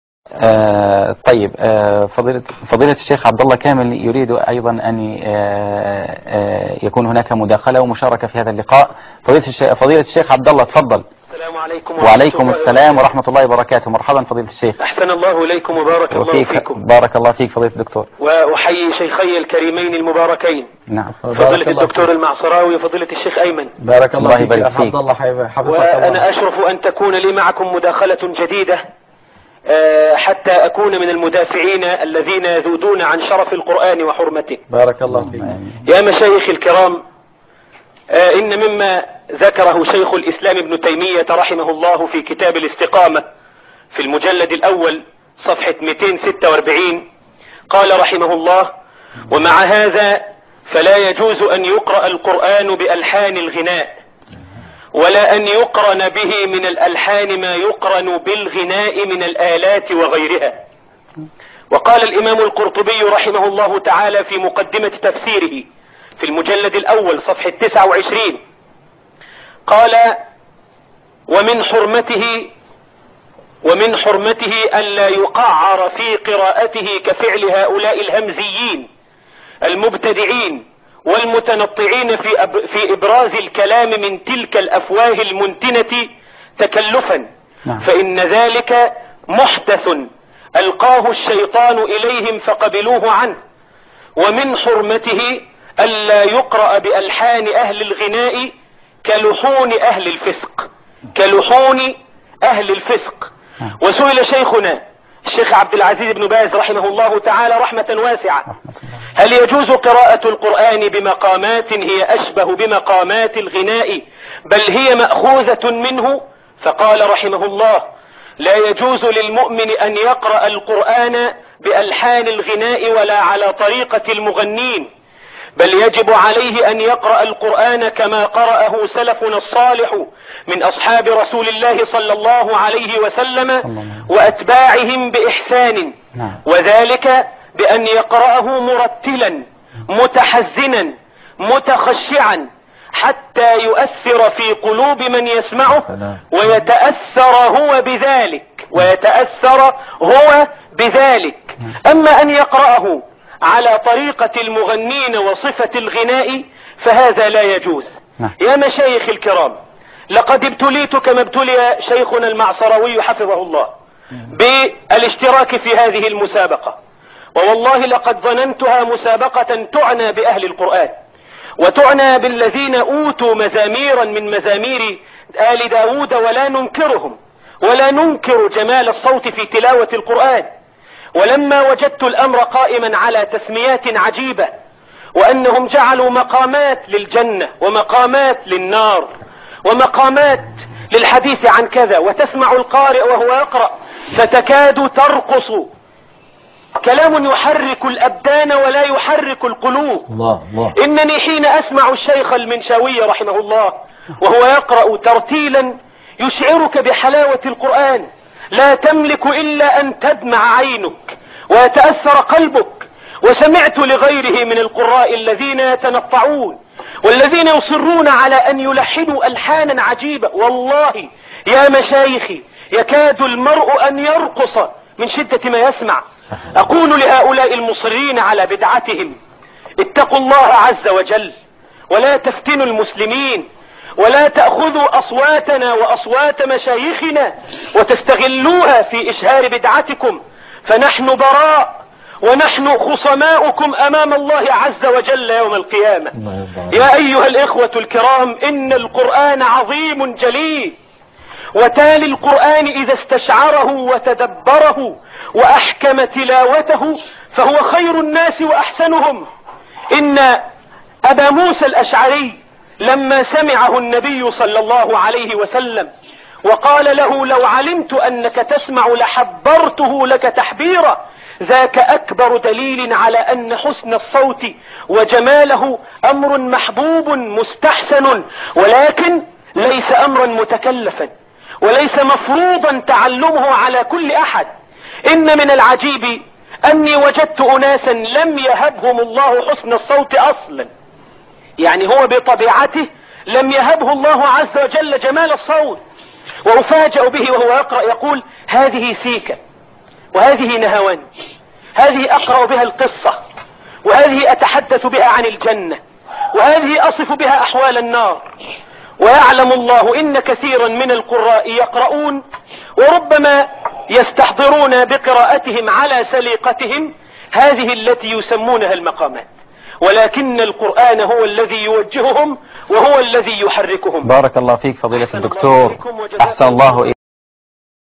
الشيخ عبد الله كامل والمقامات الصوتية في القرآن الكريم
دمـوع الشيخ أحمد عـيسى المعـصـراوي عـند كـلمات الشيخ عـبد الله كامـل حفظه الله حول بدعة المقامات الصوتية في القـرآن الكـريـم.